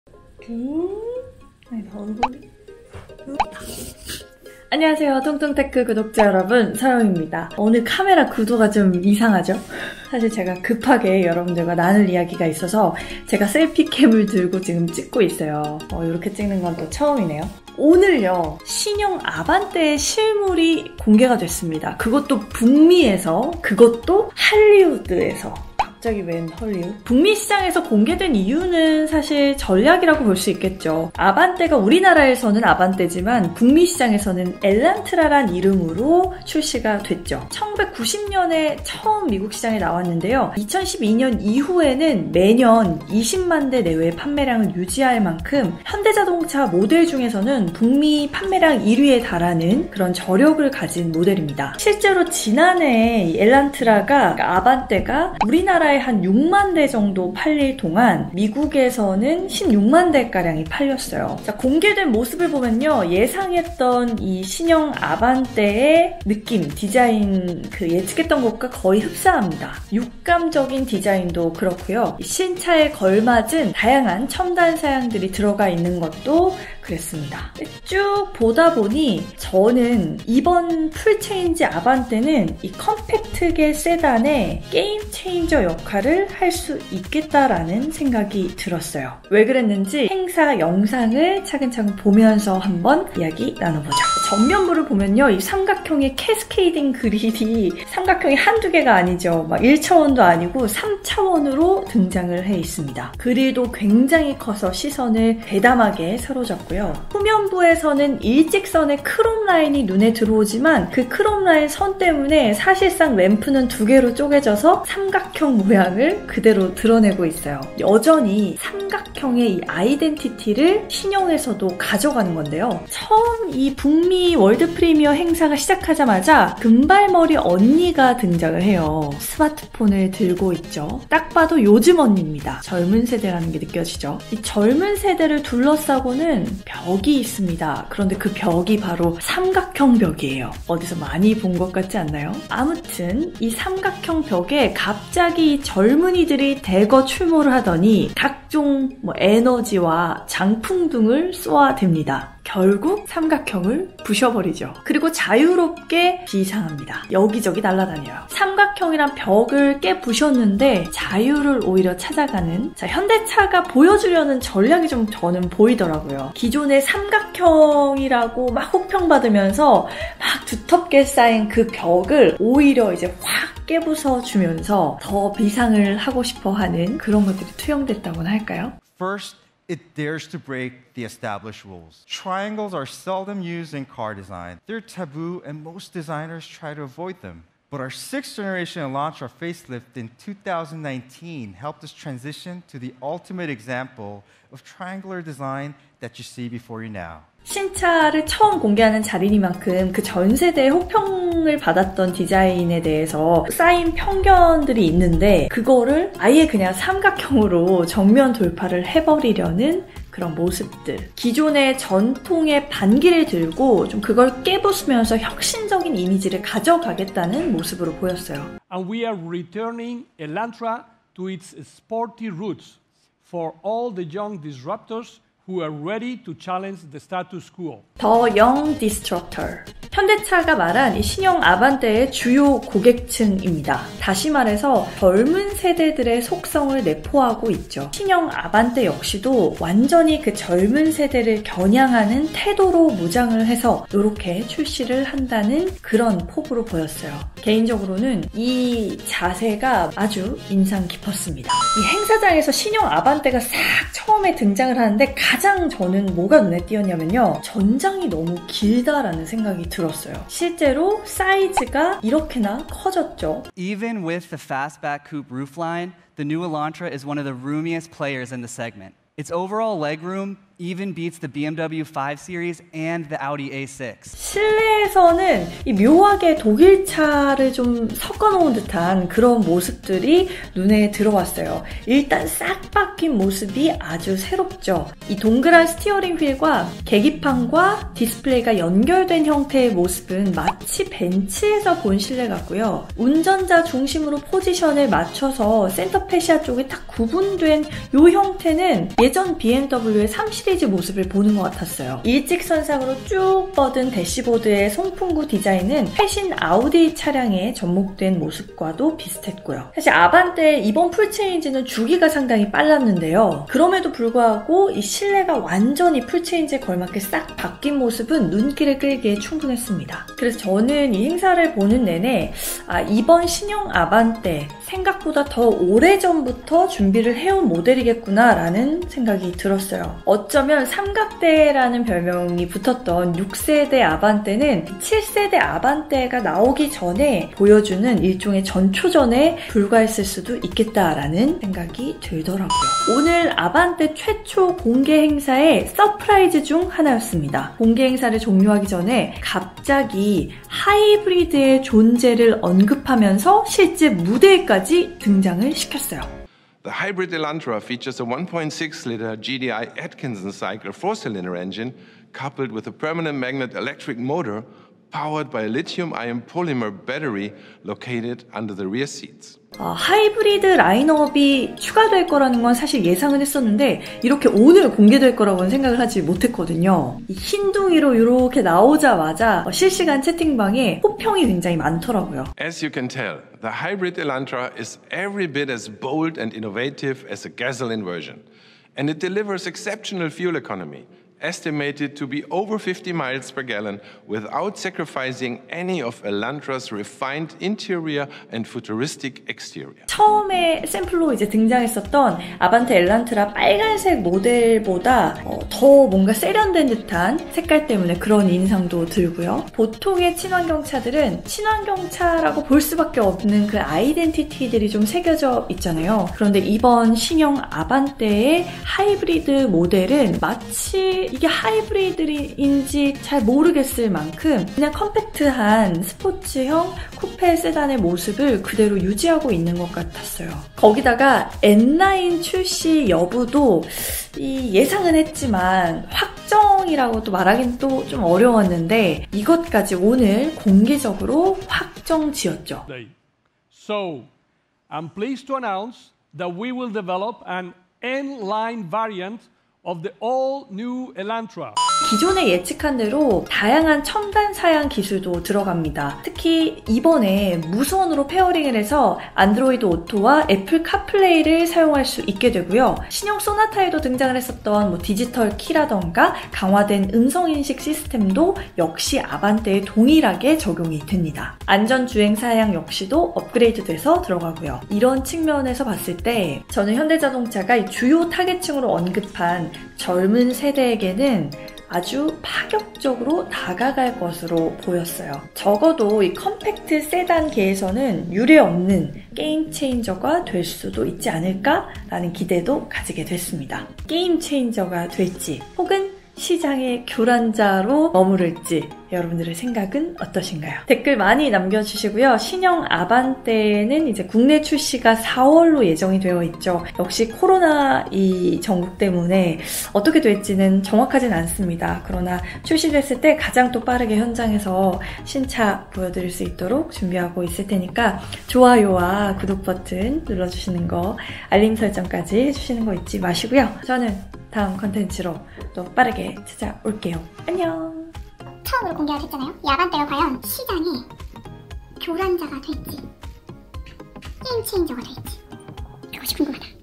(ENG) 풀체인지 아반떼, 신형 실물 공개! 컴팩트 세단계 ‘게임체인저’ 될까? (N라인, 하이브리드, 최초 공개, elantra, hyundai, 2021)
신형 아반떼(북미명: 엘란트라)의 실물 모습이 세계 최초로 공개 되었습니다 :)
미국 LA 헐리우드에서 열린 세계 행사 현장 생중계를 보며 든 하나의 생각...
'7세대 아반떼... 심상치 않은데?!'

여러분들의 생각도 같이 나누고 싶어서 또 빠르게 찾아왔어요!

[편집: 그게 안희라]